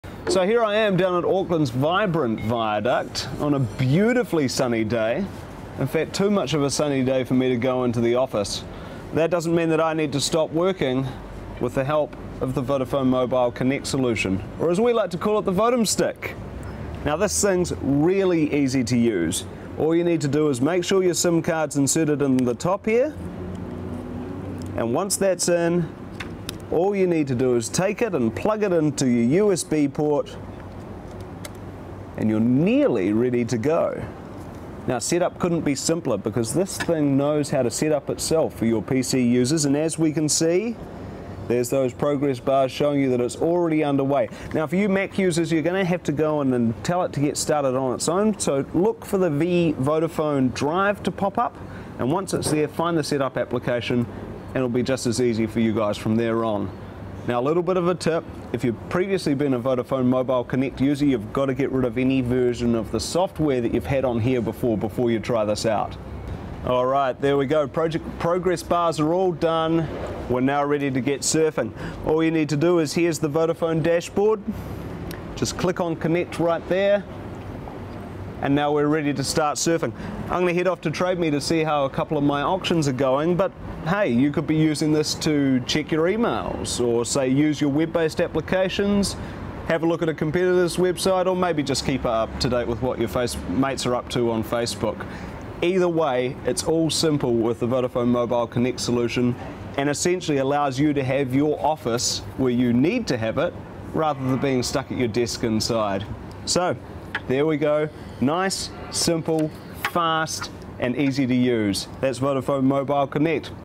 How to use a Vodem stick
Quick overview of getting online with the Vodafone Mobile Connect solution - otherwise known as a Vodem.